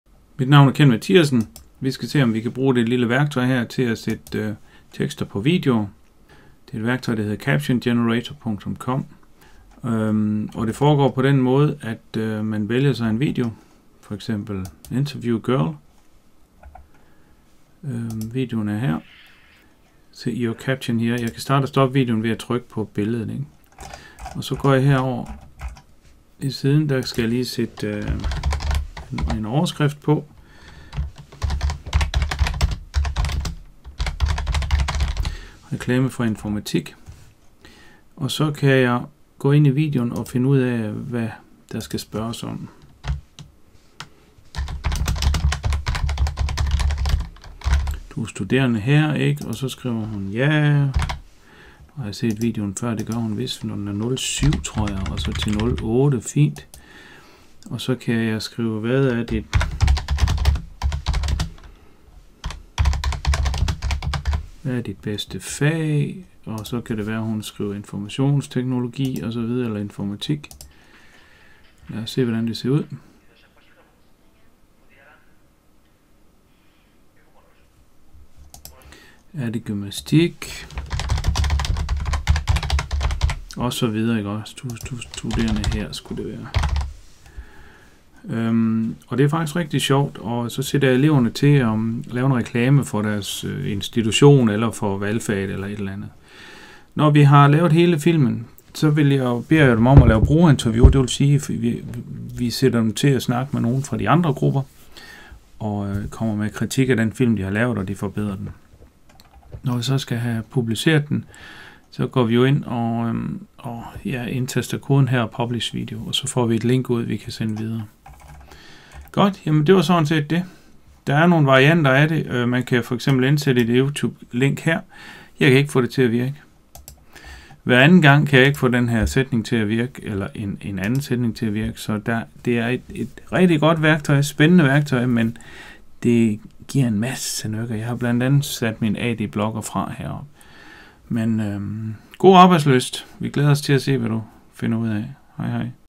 Fagdidaktisk Caption Generator
Quicky - Caption Generator. Fagtidaktisk kursus infomatik (gymnasielærer)
Kort introduktion til et værktøj hvor man kan sætte tekster på videoer.
Eleverne synes det er sjovt og det giver mulighed for at afprøve udviklingsmodellen (idé, konstruktion, test, brugertest osv)